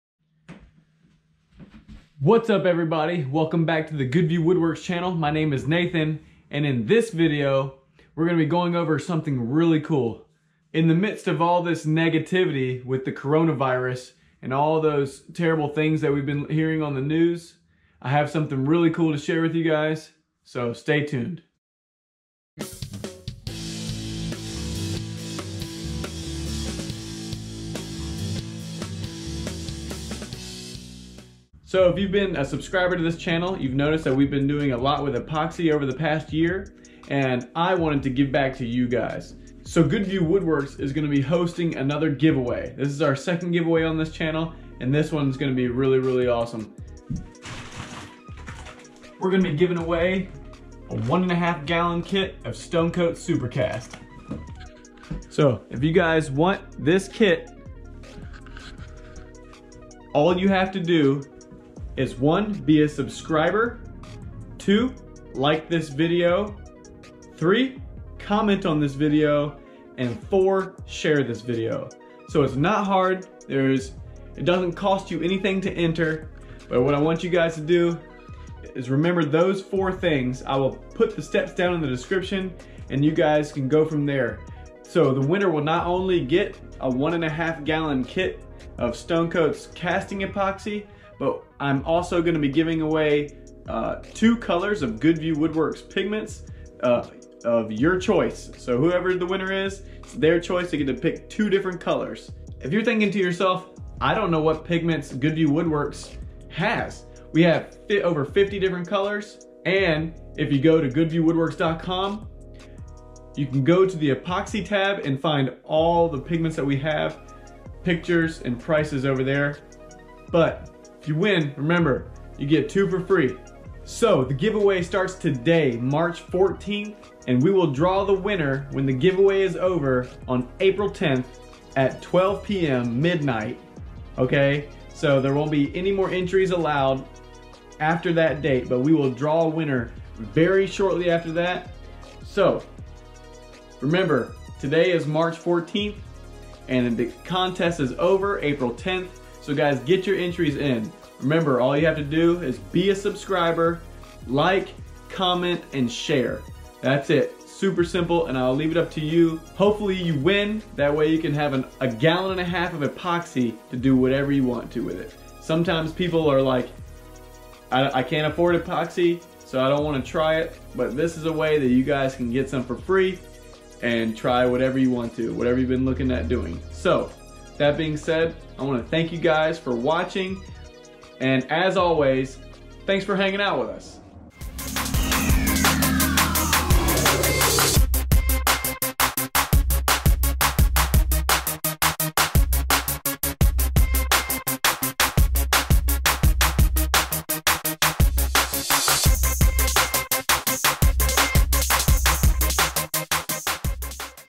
Casting Epoxy Giveaway!!! Win Stone Coat Casting Epoxy!!!
We are giving away 1 Stone Coat Countertops Casting Epoxy Kit and 2 Goodview Woodworks pigments!

Rules to enter:
1. You must be a subscriber to Goodview Woodworks Youtube channel.
2. you must "like" this video by clicking the "thumbs up" button
3. you must comment on this video.

1. Eligibility: This Giveaway is open only to legal residents of the United States (excluding its territories and possessions) and Washington D.C. who are at least 18 years of age at time of entry (“Entrant"(s)). Void in Puerto Rico, the Virgin Islands and where prohibited.

2. Prize: The Giveaway allows Entrants to enter random drawings, as described below, on the Goodview Woodworks YouTube Channel for a chance at a 1.5 Gallon Kit of Stone Coat Countertops Casting Epoxy. The average retail value (“ARV”) of the Prize is $150. The total ARV of all Prizes is $150. THE ODDS OF WINNING THE PRIZE DEPENDS ON THE TOTAL NUMBER OF ELIGIBLE ENTRIES RECEIVED.

No Prize substitution or cash alternative is allowed except at the sole discretion of Sponsor. If a Prize cannot be awarded due to circumstances beyond the control of Sponsor, a substitute prize of equal or greater retail value may or may not be awarded. The Winner will be solely responsible for all federal, state and/or local taxes, and for any other fees or costs associated with the Prize received.
3. Eastern Time ("ET") on Saturday March 14th, 2020 at 3:15pm and shall end at 11:59 p.m. ET on Friday April 10, 2020).

5. Winning:  The winner of the Giveaway (“Winner”) shall be chosen by the Sponsor based on random drawing of valid entries. The Winner shall be chosen within 2 days of the end of the Giveaway Period.

6. Prize Claim: The Winner will be notified within 5 days of the drawing via Youtube and Email. The Winner may be required to execute an Affidavit of Eligibility, a Liability Release, and (where legal) a Publicity Release within 7 of notification, or as directed by the Sponsor. The failure to respond timely, to the notifications and the failure to provide the Prize Claim Documents will result in forfeiture of the Prize and, in such case, the Sponsor may declare an alternate winner, based on another random drawing. If a Prize goes unclaimed or is forfeited by the Winner, the Prize may not be re-awarded, in Sponsor’ sole discretion. The Winner may be required to provide Sponsor with a valid social security number before the Prize will be awarded for tax reporting purposes.  An IRS Form 1099 may be issued in the name of the Winner, for the actual value of the Prize received. Other restrictions may apply.

6. Rights of Sponsor: Sponsor, shall have the right, at its sole and absolute discretion to refuse any Entry whatsoever made by Entrants which it finds to be in violation of these Official Rules or which it finds in its sole discretion to be otherwise objectionable for any reason.
7. YouTube Policies

* Your participation in and entry into this giveaway must be in compliance with YouTube’s own Community Guidelines. Entries which don’t comply with YouTube’s Community Guidelines will be disqualified.